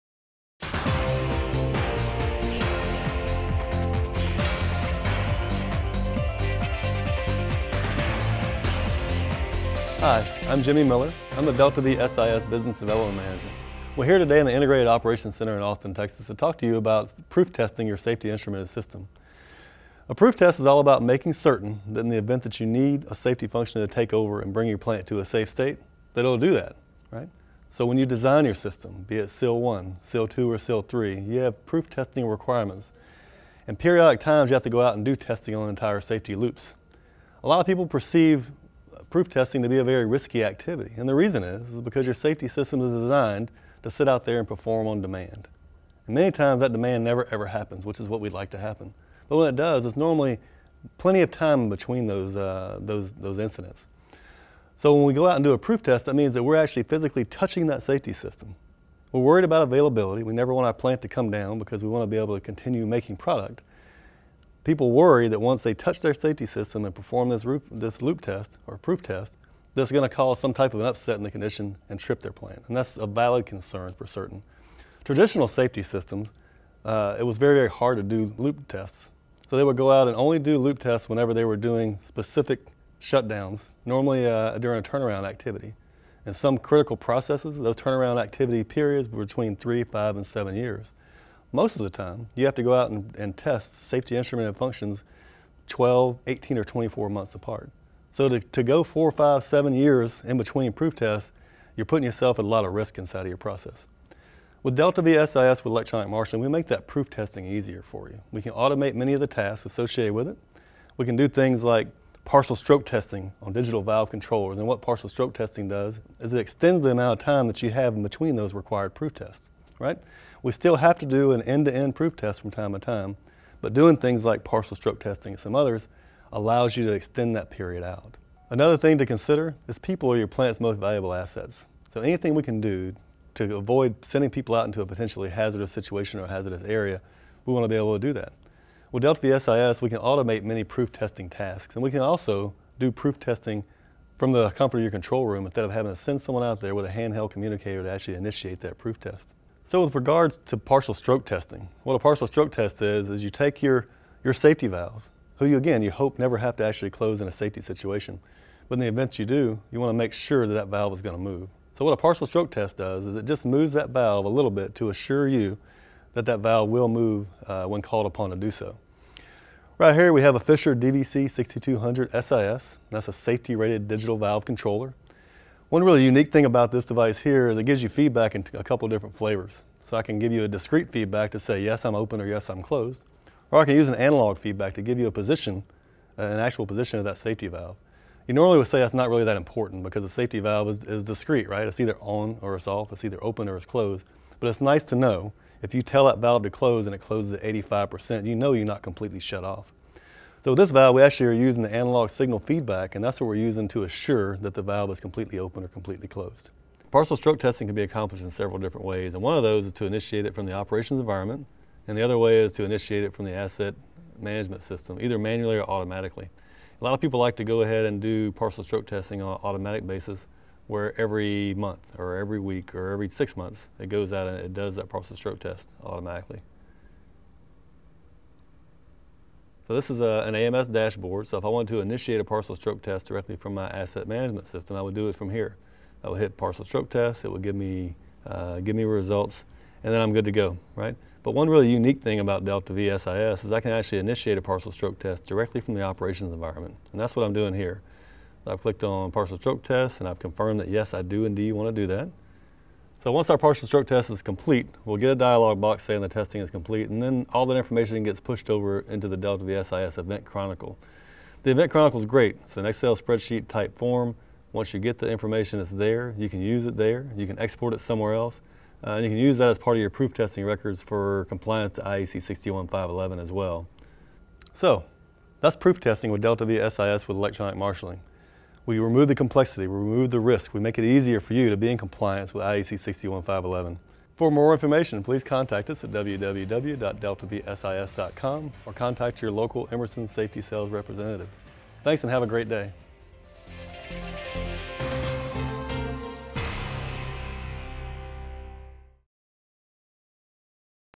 DeltaV SIS: Proof Testing
A demonstration of how DeltaV SIS with Electronic Marshalling and partial stroke tests can deliver safer, more efficient proof testing.